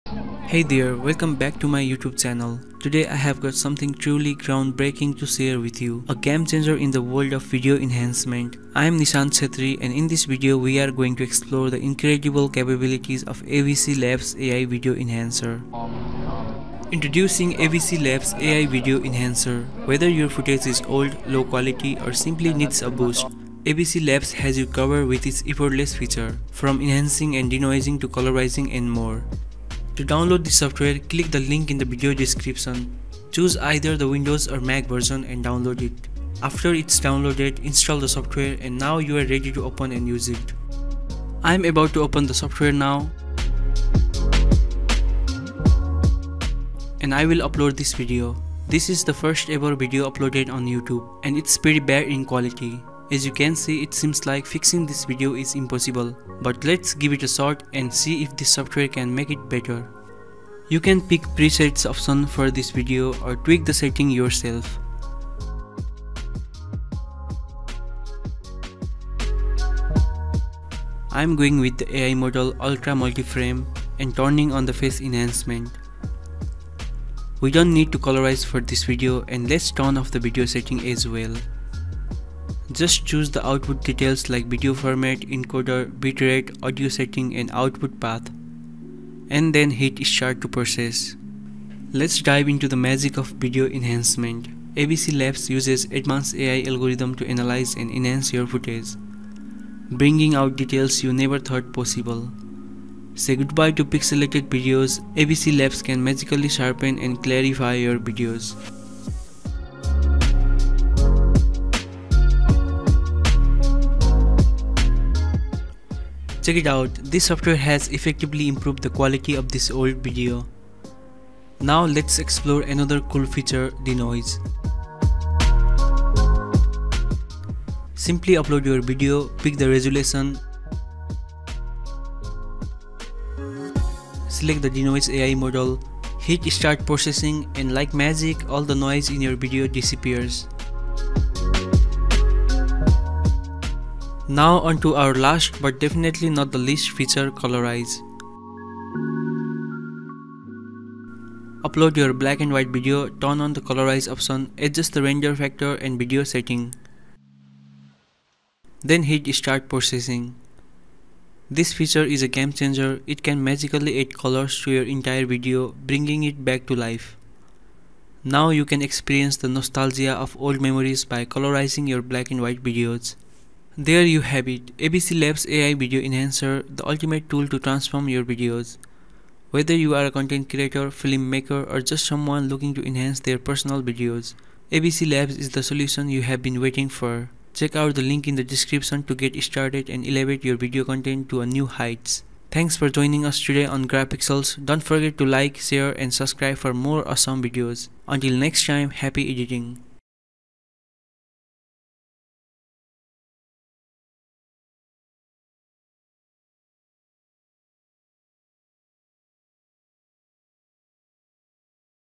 This Software Can Upscale Your video Quality Upto 8k!
🎥 Video Overview:🚀 Welcome back! Introducing AVCLabs AI Video Enhancer – a game-changer for old or low-quality footage. Elevate your videos with a few clicks! 🌟 Download for Windows or Mac in the description. Transform the first YouTube video – can AVCLabs improve its quality? 🎨 Customize settings, denoise, and colorize for a vibrant experience. 👉 AVCLabs: Ultimate tool for creators. Thanks for joining – happy editing! ✨